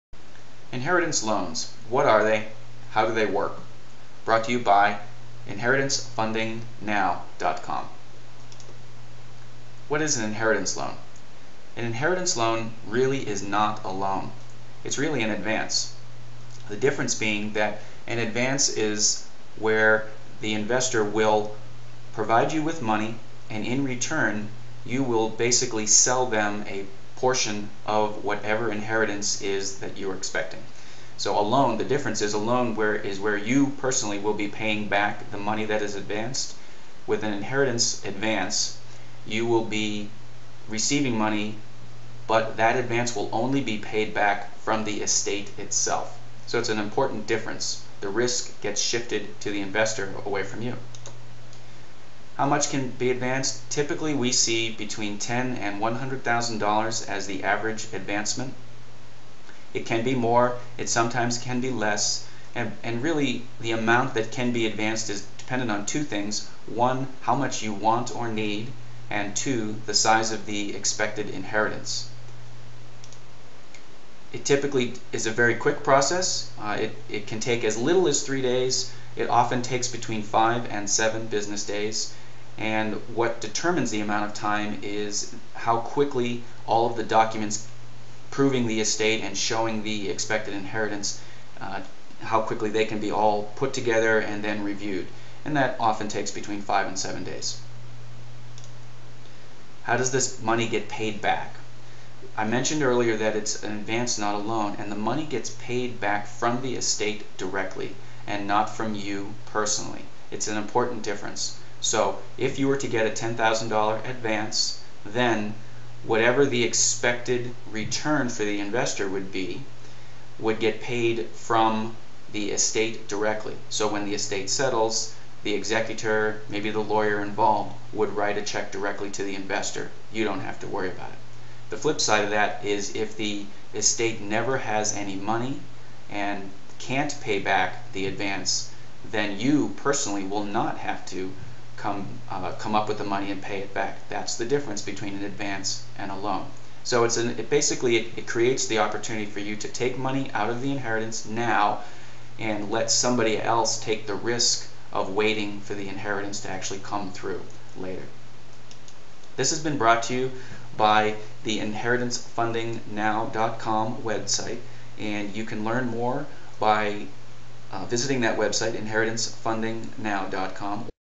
Inheritance Loans: What Are they
Inheritance Loans are basically cash advances. They get paid back from the estate itself.